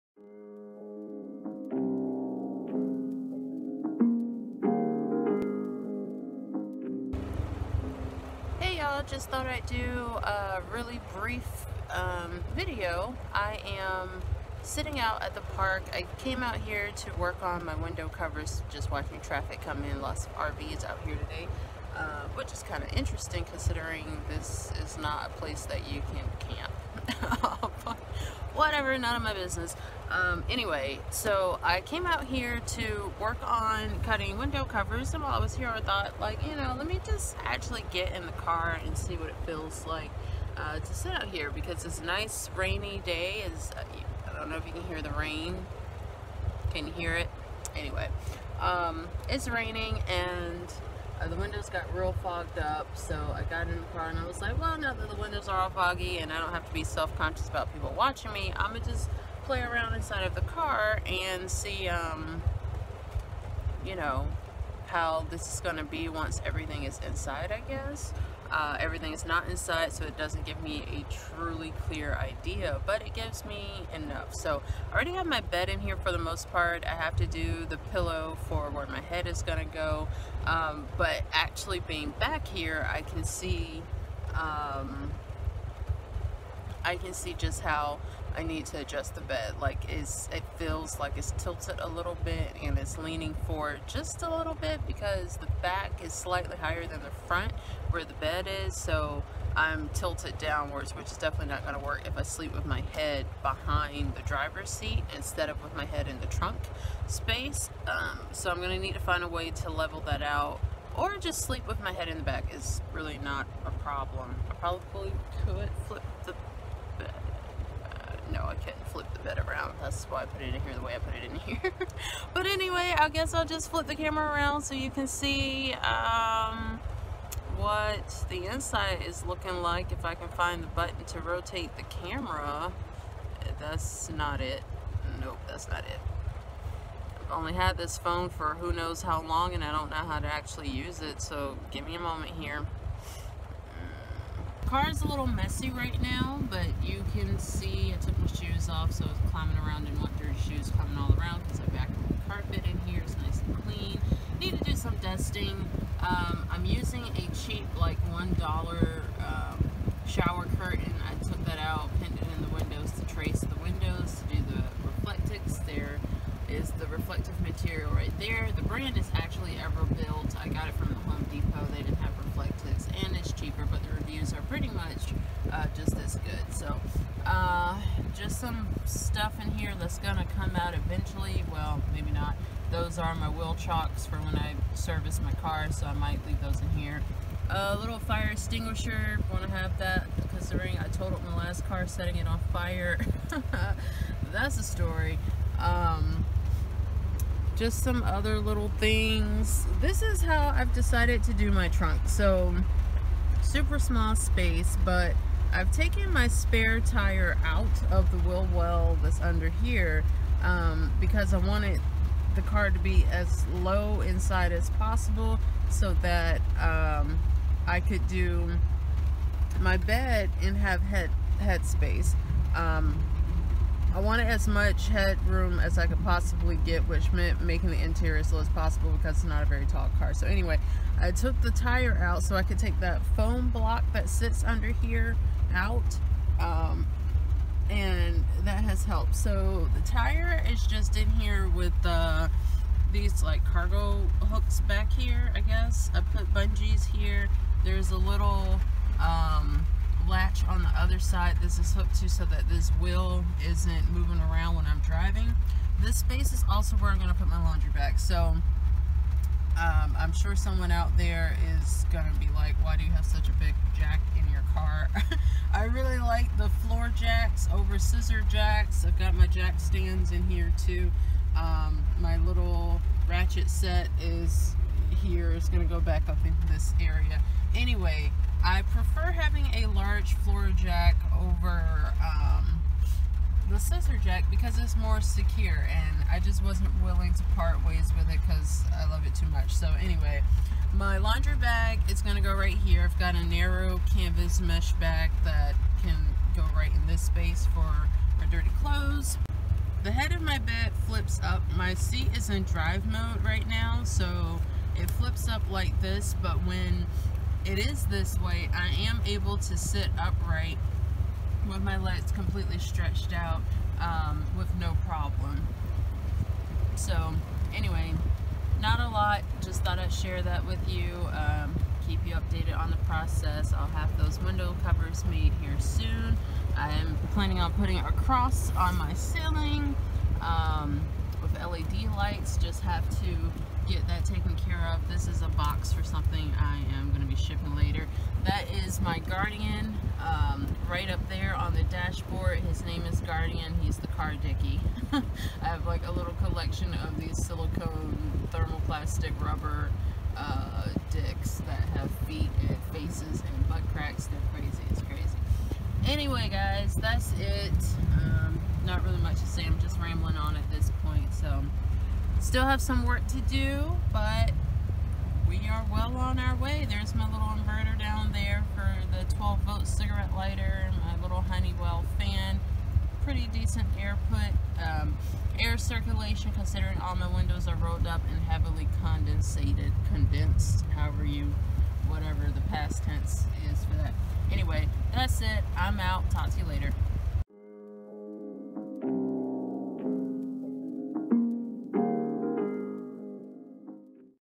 Mini Car Progress UPDATE ~ Car Life ~ Vehicle Dweller
Hey ya'll, just a brief update on my plans for the car and testing out my space while hanging at the park on a rainy day.